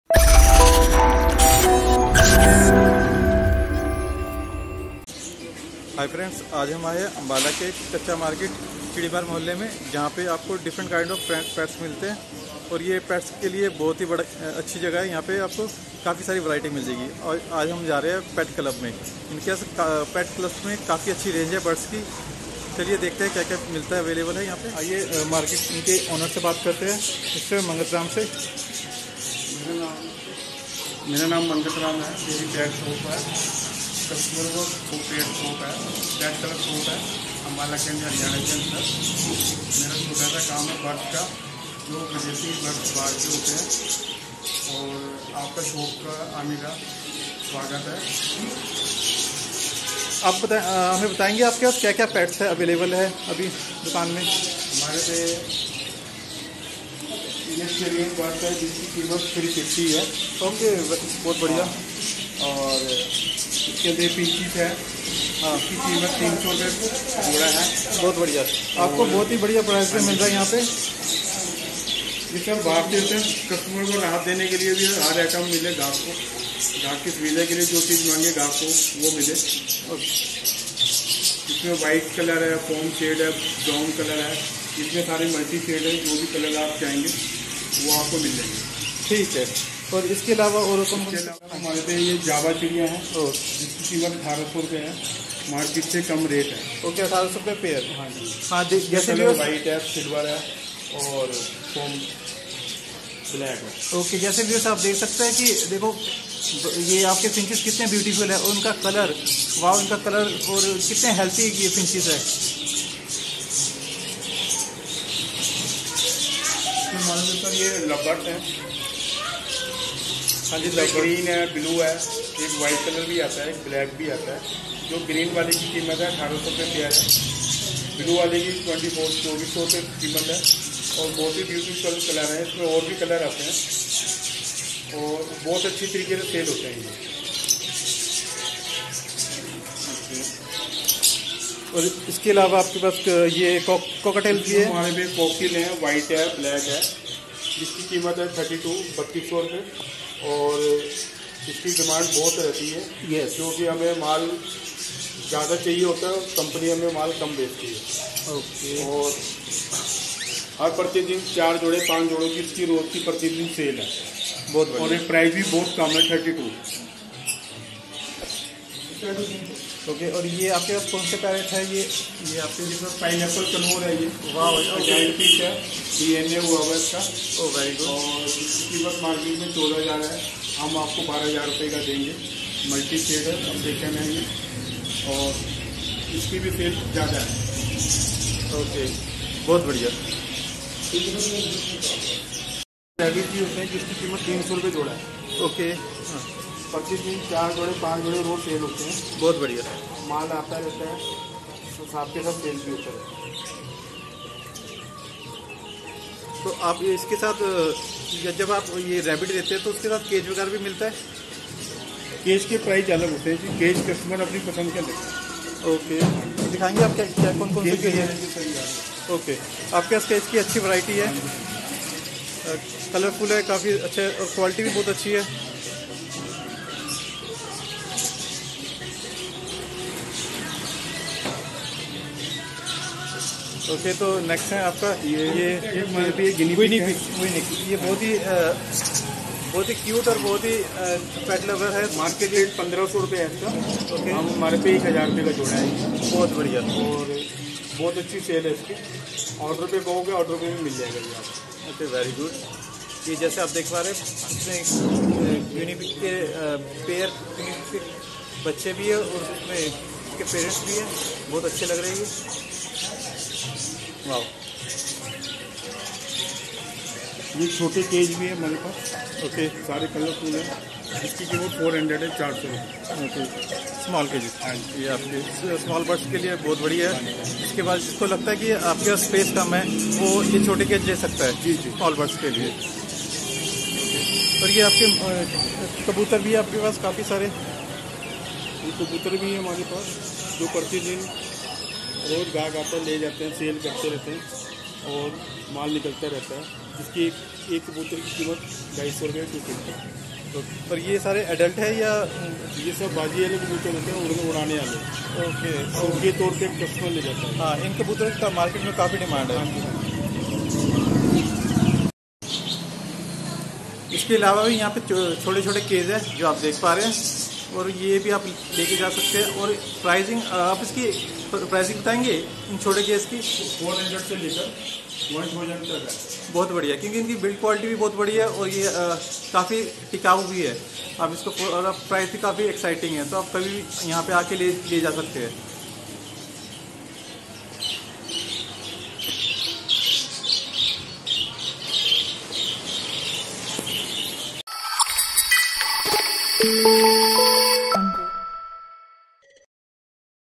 Birds Shop In Ambala | exotic birds
Shop Owner - Mangat Ram
Shop Name - Pet club
Address:- 2183, kacha bazar, Chirimar mohalla Ambala cantt 133001

for any query you can comment on us
Thanks 🙏😊

 Correction-  Kindly noted down .Pine conure price is 12000Rs with DNA test not 1200Rs.

Description

Hi Friends this is first vlog of Bids shop in Ambala cantt. now you can buy exotic birds at your city. they have cockatiel parrot, budgies, love birds, finches, Guinea pigs, rabbits, pigeon and many more pets. they provide you in good price range.
 Don't call for ring neck or Alexander parrots. they don't deal with illegal bird's.